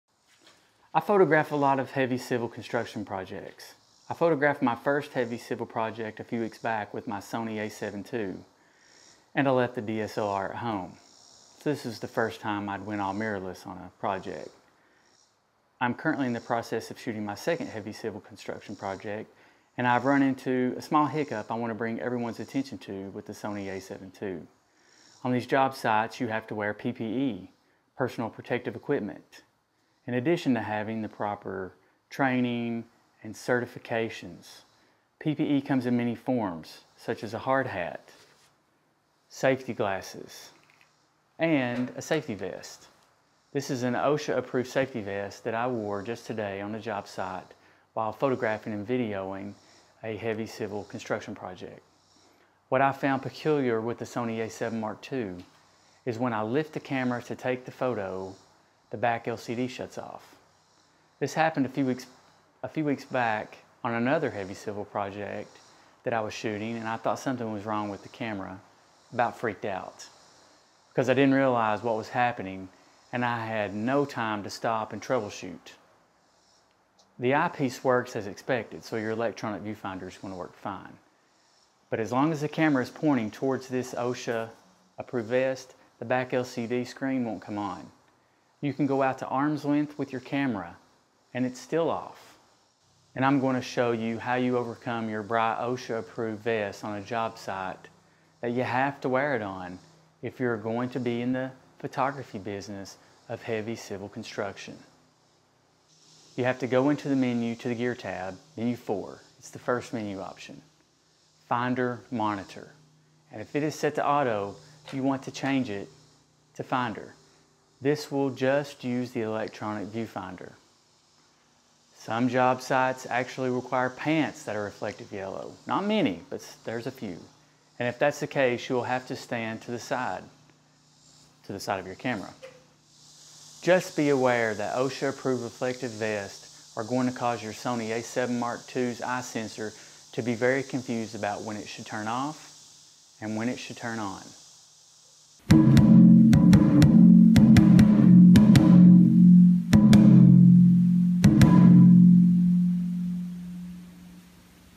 OSHA Safety Vest Confuses Sony a7ii Eye Sensor
OSHA safety reflective vests confuse the Sony a7 series cameras eye sensor and shut the back LCD screen off. It is good information to know that reflective clothing can cause this problem while you are arm is fully extended in front of you. This video shows the issue and what you should expect and what you can do to sidestep the issue.

The a6000 focus acts up 1:37